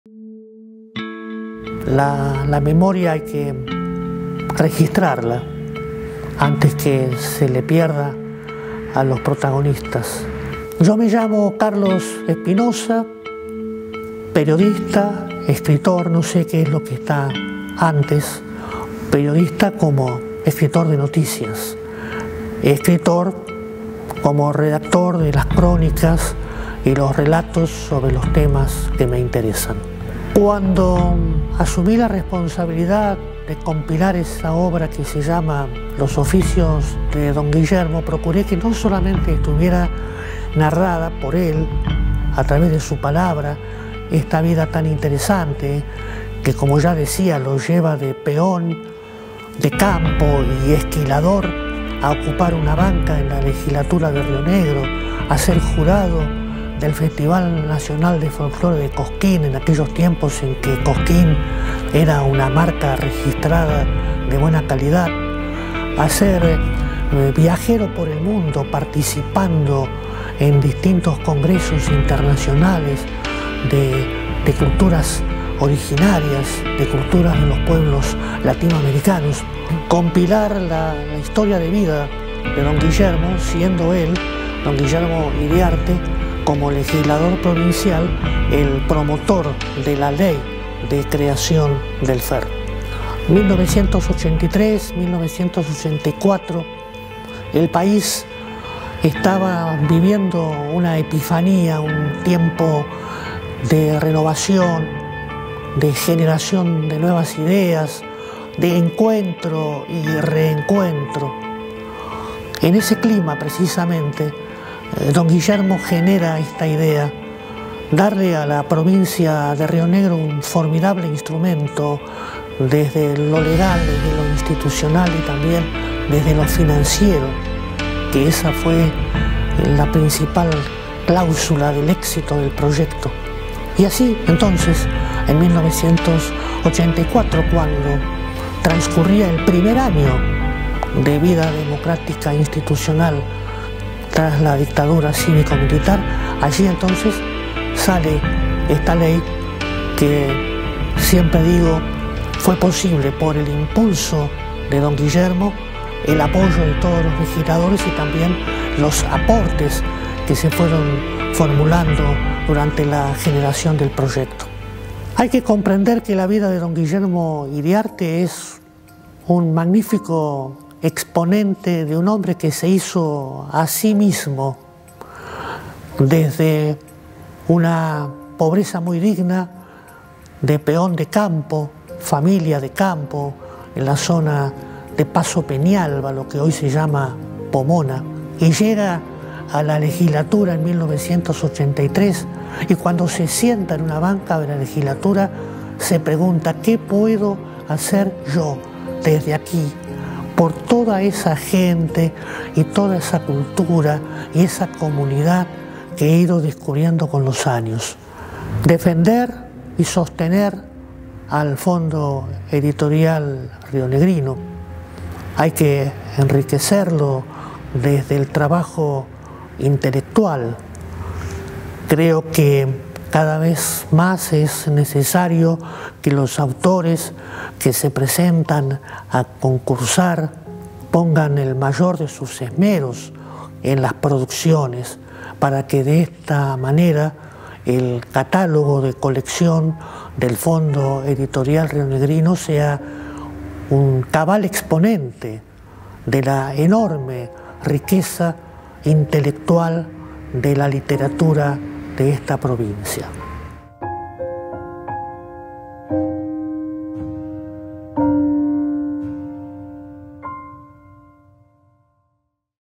Fondo Editorial Rionegrino, 40 años
Carlos Espinosa recuerda el momento de fundación del Fondo Editorial Rionegrino hace 40 años.